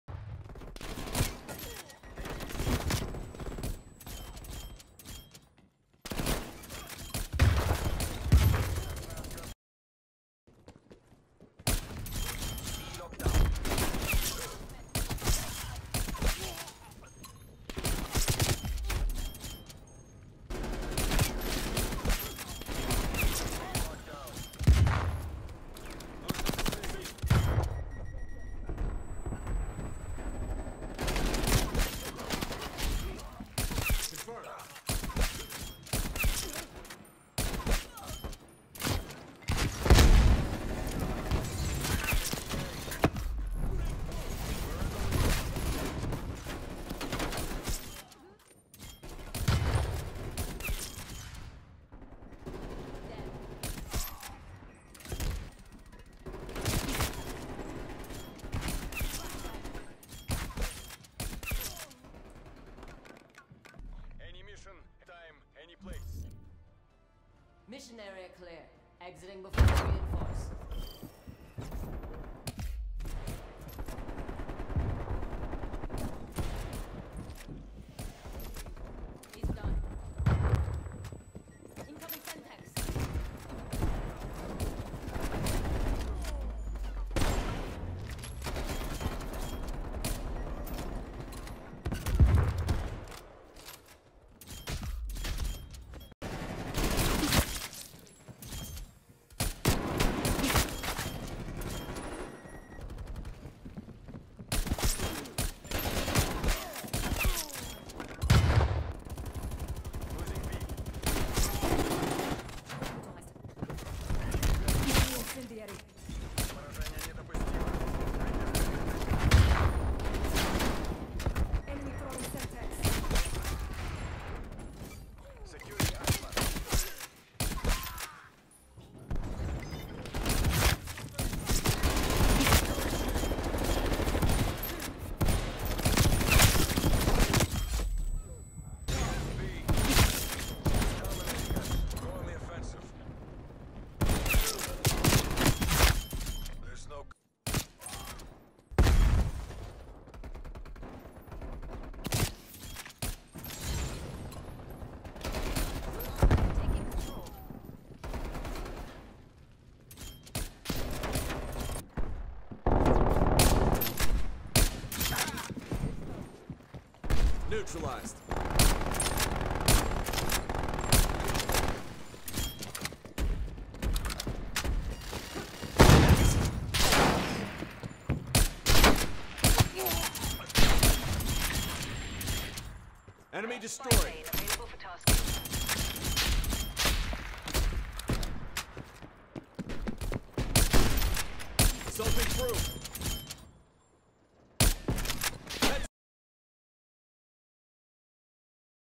using IRON SIGHT on SWISS is CHEATING (Best Class Setup/Class)
Time Stamps
Intro Clip 0:00 - 0:10
Gameplay/Clips 0:12 - 3:15
Class Setup 3:16 - 3:19